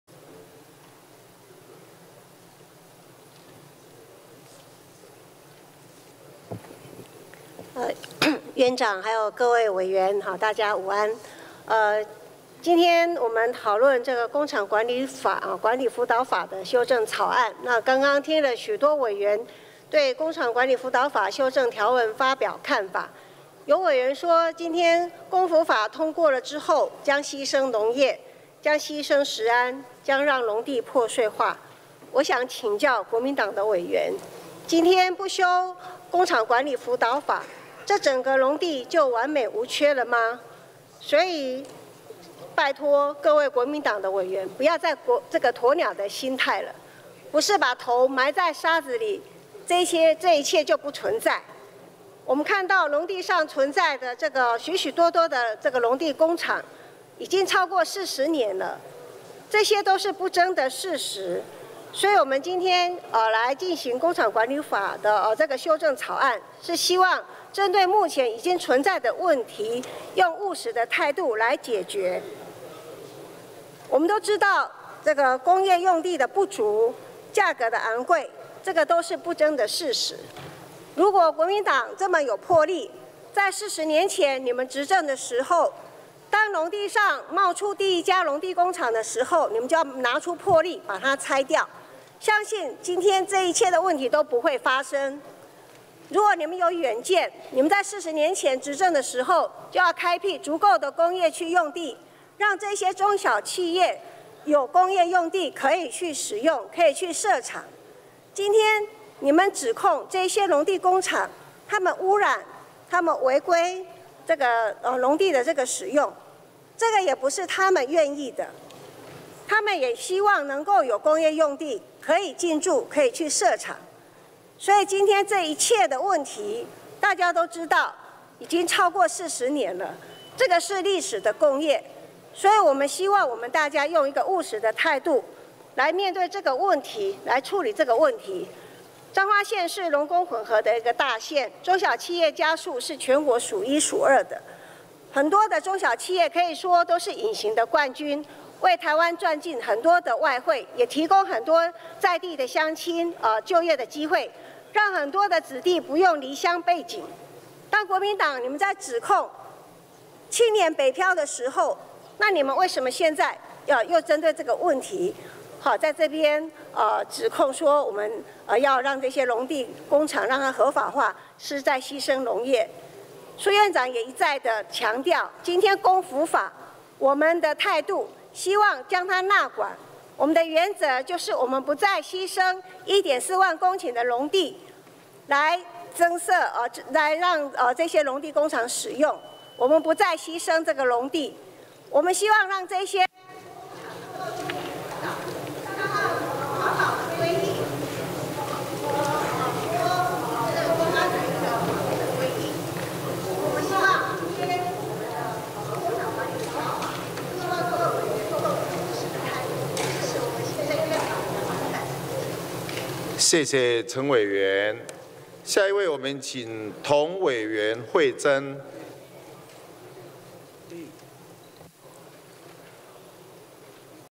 工輔法 三讀 24 陳素月 (民進黨/彰化) 工廠管理輔導法 2019 06 27 院會 臨時會【立法院演哪齣？】
陳素月：（17時40分）主席、各位同仁。今天我們討論工廠管理輔導法的修正草案，剛剛聽了許多委員對工廠管理輔導法修正條文發表的看法，有委員說今天工輔法通過之後將犧牲農業、將犧牲食安、將讓農地破碎化，我想請教國民黨的委員，今天不修工廠管理輔導法，這整個農地就完美無缺了嗎？所以拜託各位國民黨的委員不要再鴕鳥心態了，不是把頭埋在沙子裡，像是這一切就不存在，我們看到農地上存在的許許多多的農地工廠已經超過40年了，這些都是不爭的事實。所以我們今天進行工廠管理輔導法修正草案是希望針對目前已經存在的問題，用務實的態度來解決。我們都知道工業用地不足以及價格昂貴都是不爭的事實，如果國民黨這麼有魄力，在40年前你們執政時，當農地上冒出第一家農地工廠，你們就要拿出魄力把它拆掉，相信今天這一切的問題都不會發生；如果你們有遠見，你們在40年前執政時，就要開闢足夠的工業區用地，讓這些中小企業有工業用地可以使用、可以設廠。今天你們指控這些農地工廠污染以及違規使用農地，這也不是他們願意的，他們也希望能夠有工業用地可以進駐、可以設廠。

今天這一切的問題，大家都知道已經超過40年了，這是歷史的共業，所以我們希望大家用一個務實的態度來面對以及處理這個問題。彰化縣是農工混合的大縣，中小企業家數是全國數一數二的，很多的中小企業可以說都是隱形的冠軍，為臺灣賺進很多的外匯，也提供很多在地鄉親就業的機會，讓很多子弟不用離鄉背井。當時國民黨指控青年北漂，現在你們在這裡為什麼又針對這個問題指控我們要讓這些農地工廠合法化是在犧牲農業？蘇院長也一再強調，針對工輔法，我們的態度是希望將他們納管，我們的原則就是不再犧牲1.4萬公頃的農地來增設、讓這些農地工廠使用，不再犧牲農地，我們希望讓這些工廠能夠就地輔導，讓他們能符合環保的規定以及符合許多公安的法令規定。對於今天的工廠管理輔導法，希望各位委員能用務實的態度，支持行政院版的版本，謝謝。